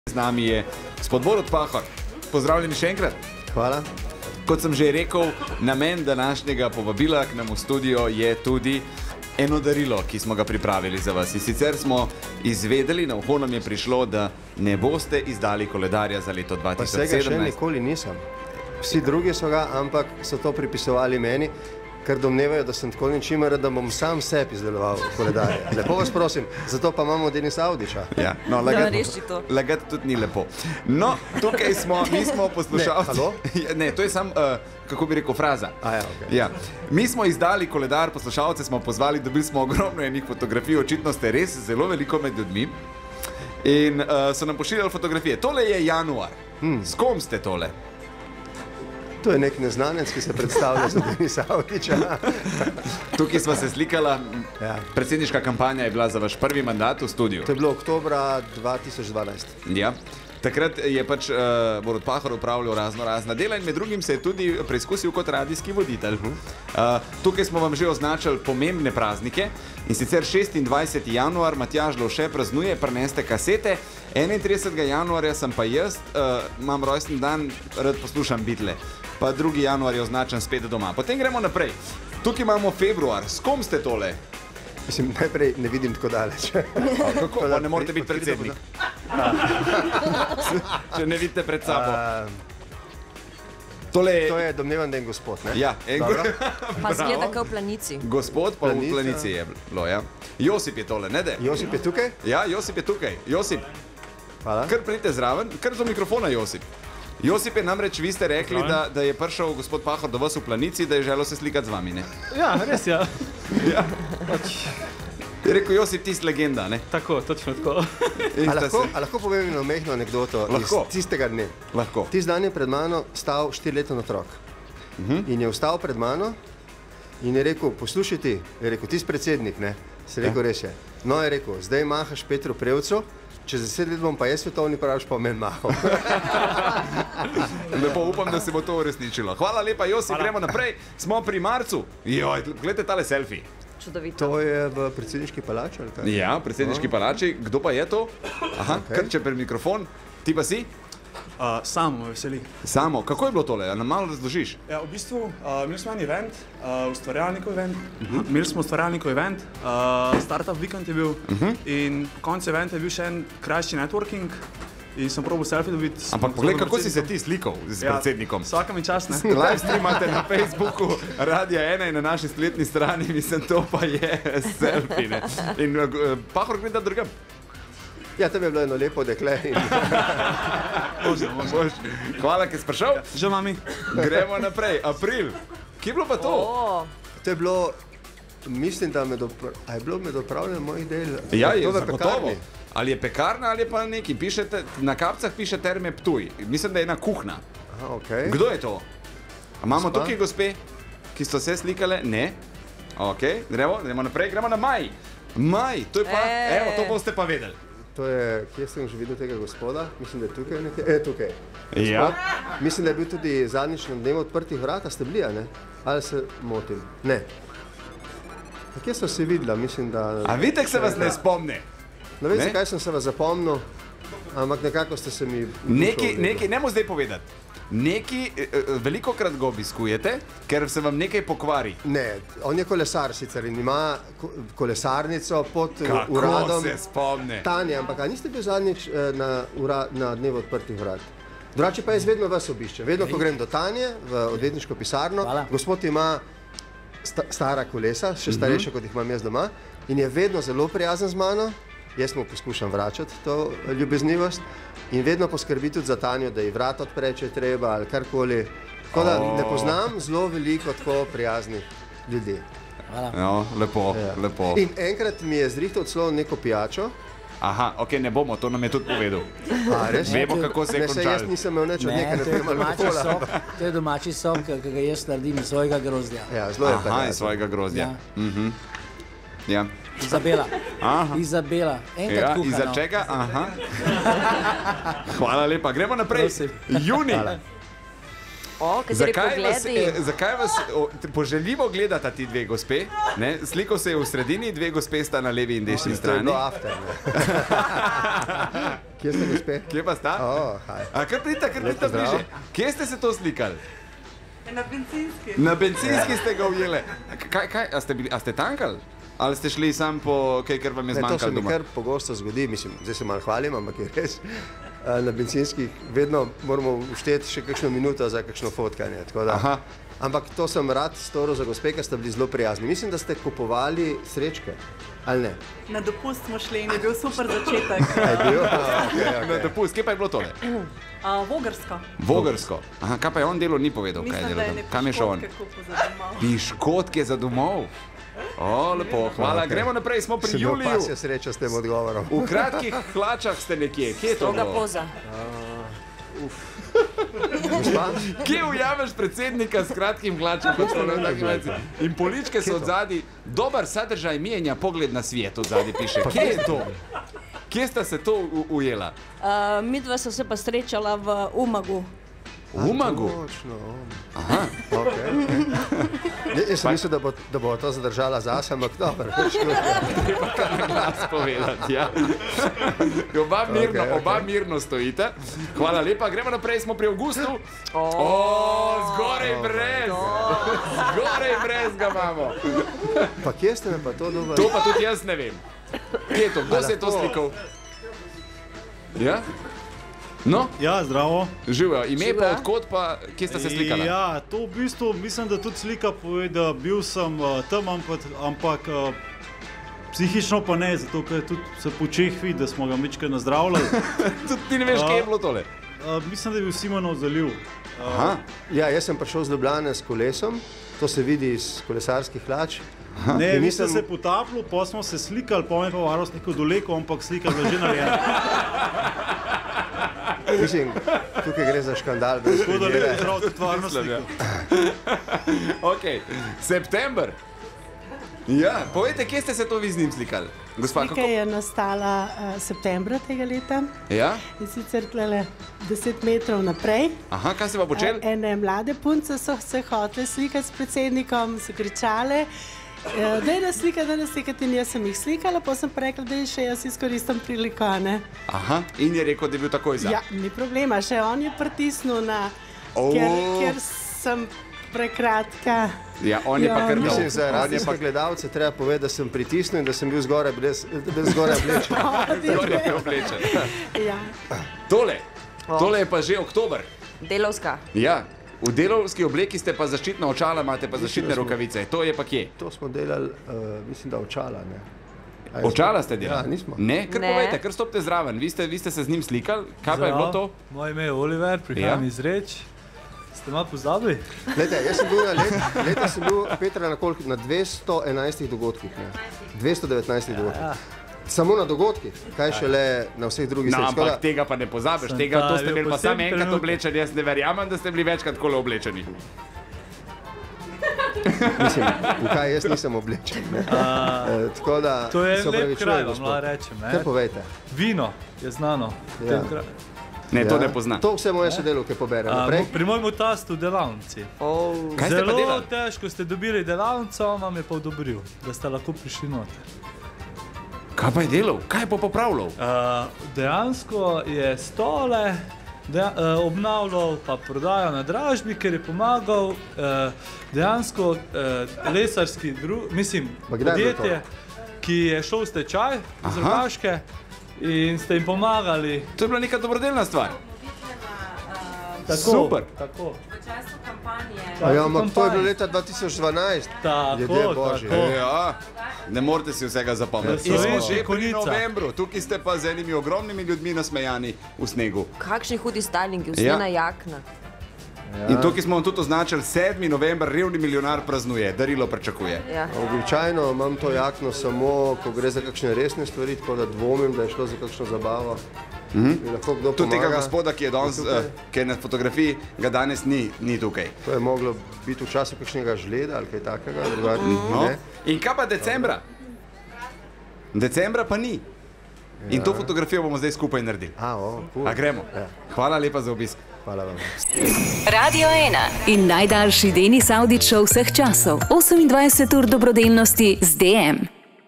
Borut Pahor: ''Hvala Denis, da ste me povabili in ker se vedno hecate na moj račun!''
Obiskali so nas tudi poslušalci, ki bodo s Pahorjem krasili koledar Radia 1 za leto 2017